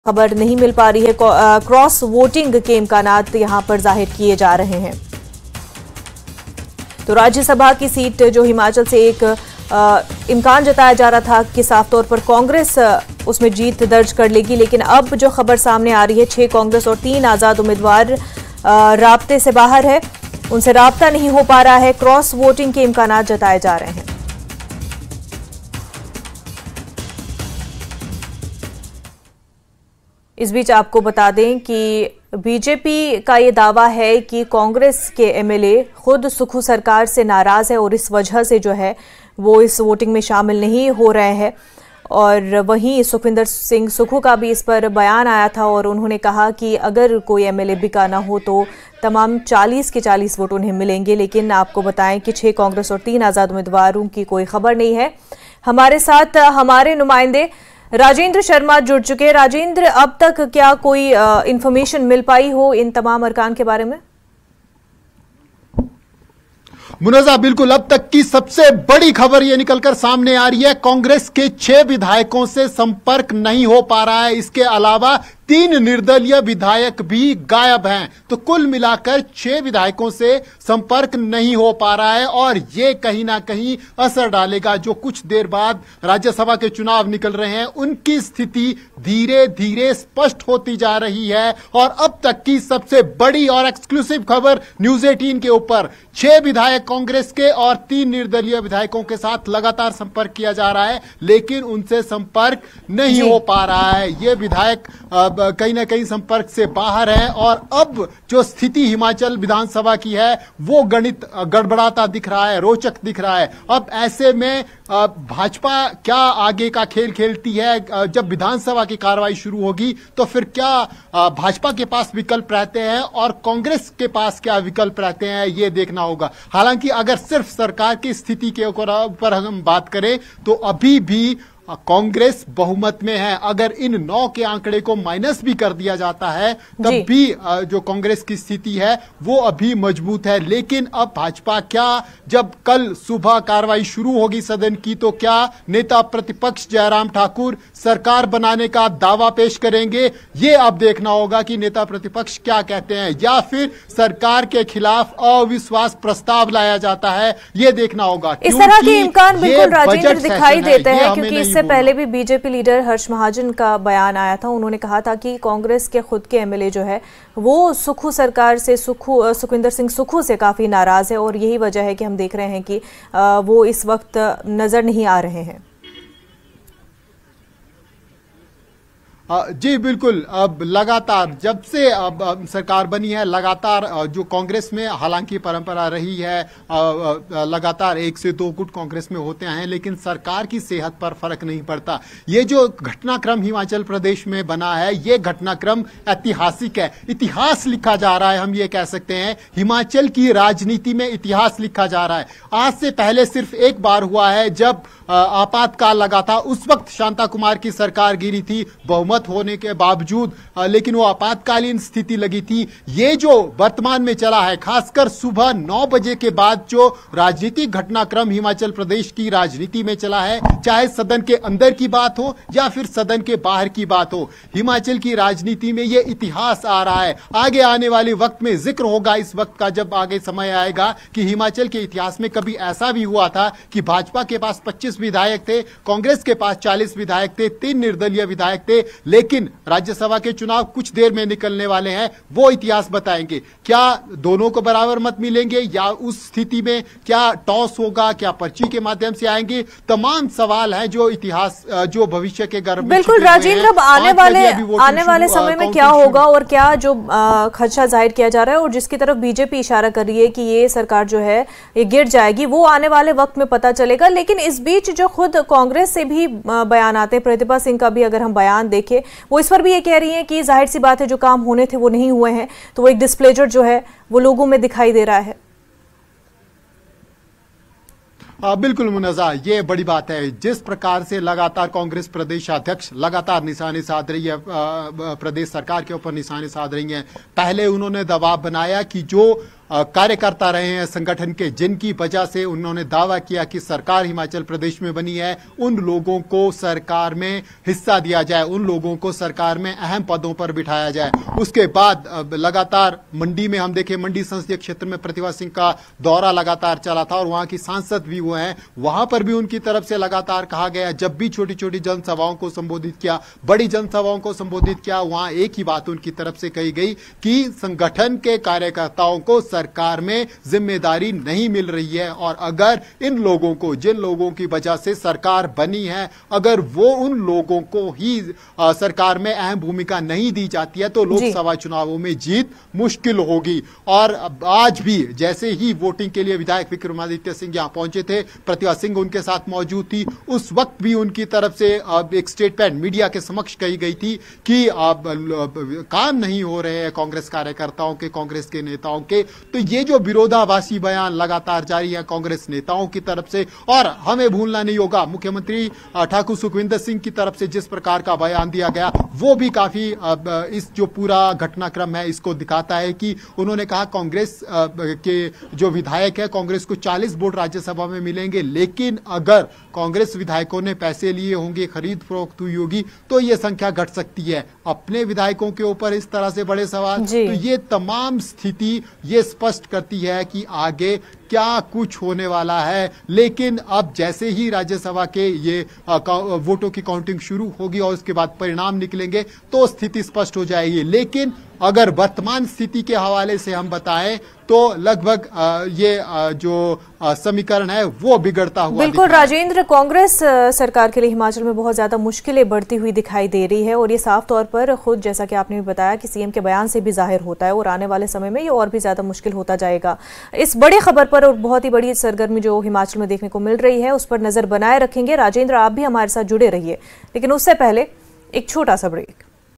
Himachal Politics | आज़ाद विधायकों समेत 9 विधायकों से लंच के बाद नहीं हो पा रहा संपर्क...
BreakingNews | सूत्रों के हवाले से बड़ी खबर
क्रॉस वोटिंग की अटकलों के बीच आज़ाद विधायकों समेत 9 विधायकों से लांच के बाद नहीं हो पा रहा संपर्क...